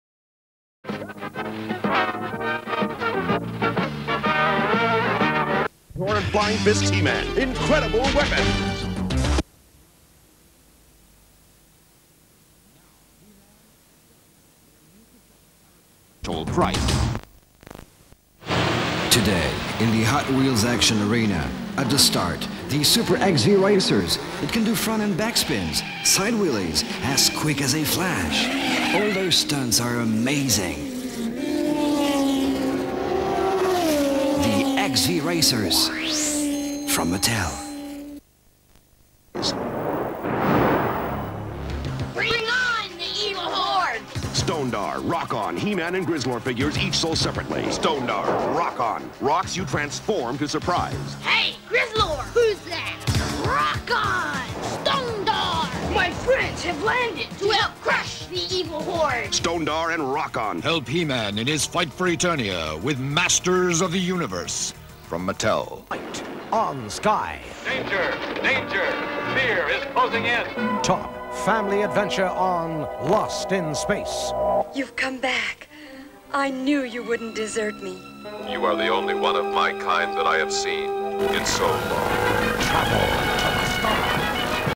A little snippet of 1980s Toy-commercials on Sky Channel
This video was not originally found or uploaded by me. But the guy who did, allows for download and reuopload of his material. So that is what this video is.